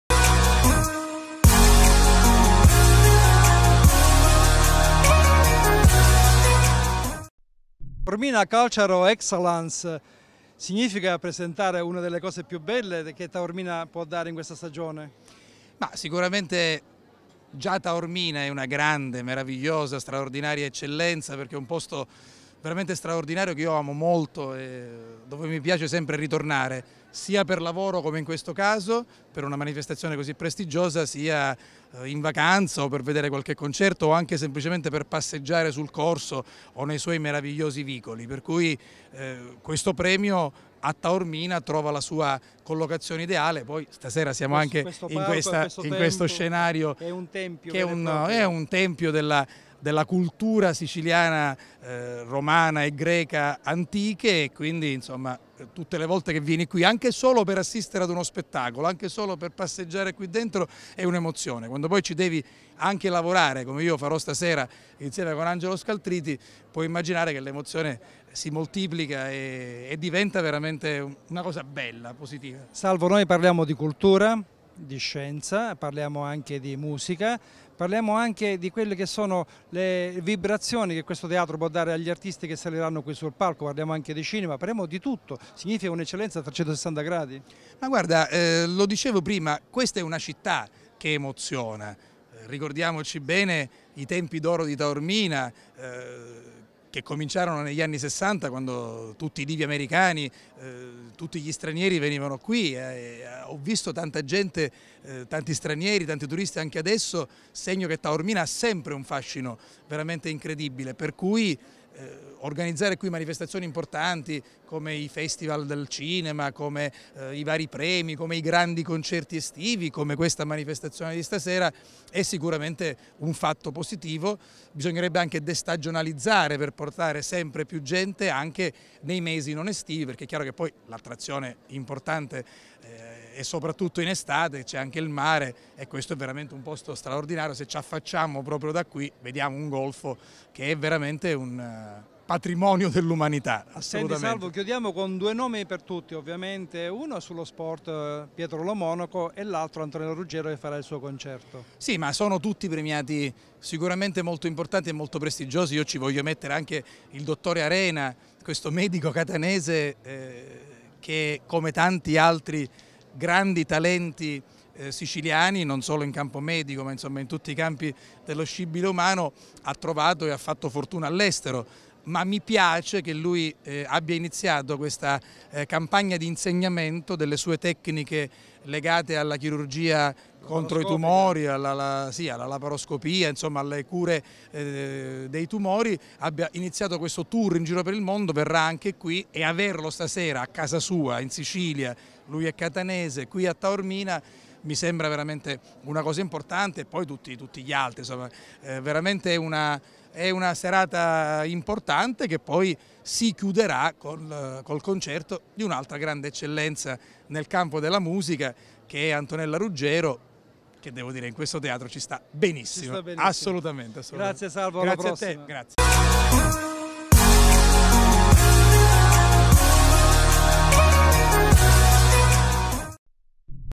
Taormina culture of excellence - Salvo La Rosa
Ecco l'intervista a Salvo La Rosa in occasione di Taormina culture of Excellence